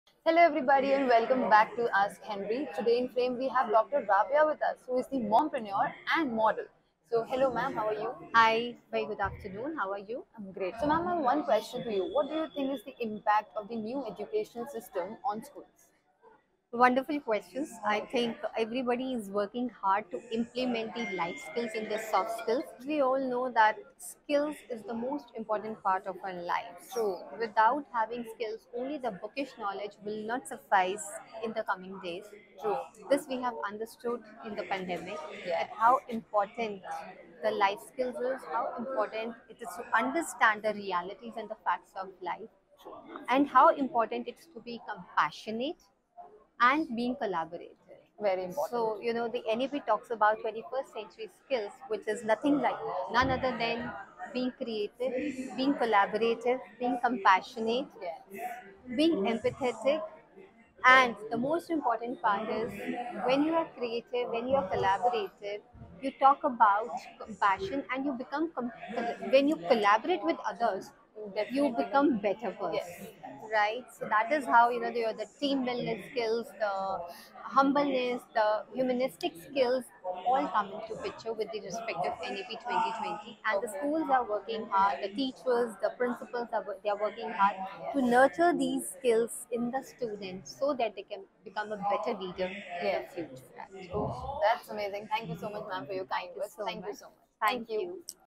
Dr. Rabiaah Bhatia Awesome Tips On Impact Of New Education Policy On Schools
Dr. Rabiaah Bhatia Awesome Tips On the Impact Of New Education Policy On Schools
Call/WhatsApp
Our 24-hour Helpline no.- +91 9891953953